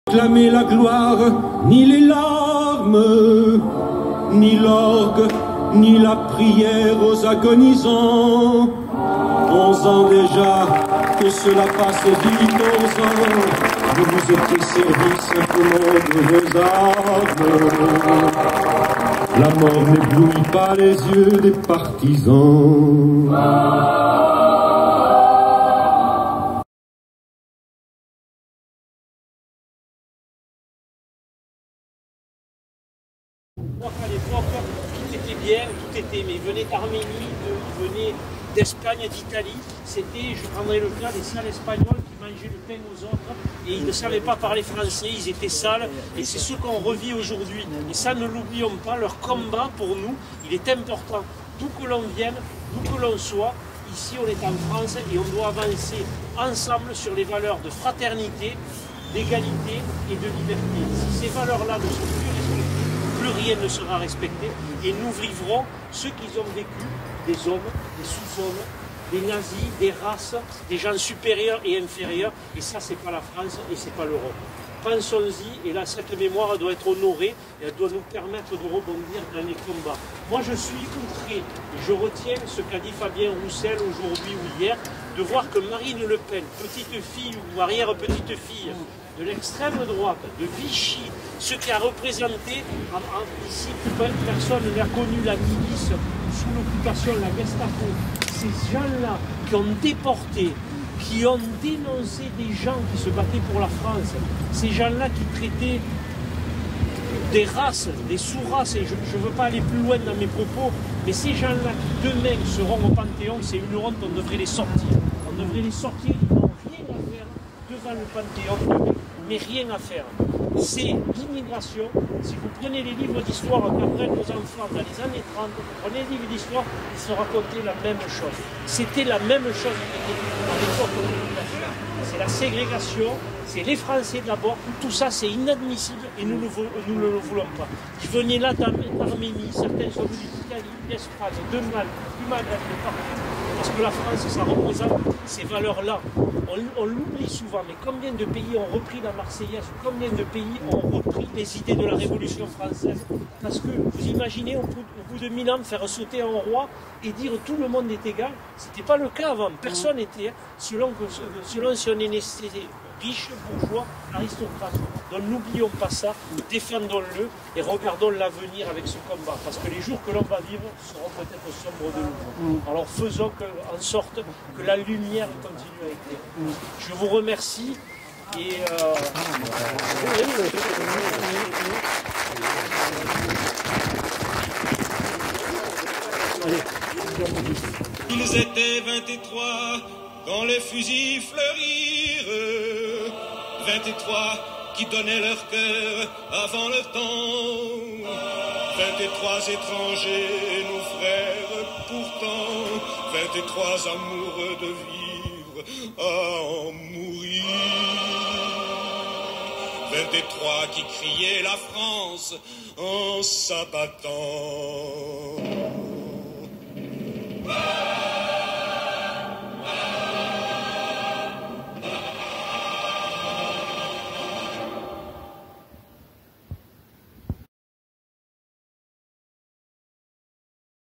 Hommage à Missak et Mélinée Manouchian, Port-de-Bouc, 20.02.2024 / 5e partie : Portrait et fleurs
Dévoilement du portrait de Missak Manouchian sur la façade de l'hôtel de ville, suivi de la pose d'une gerbe de fleurs devant la stèle rendant hommage aux résistants.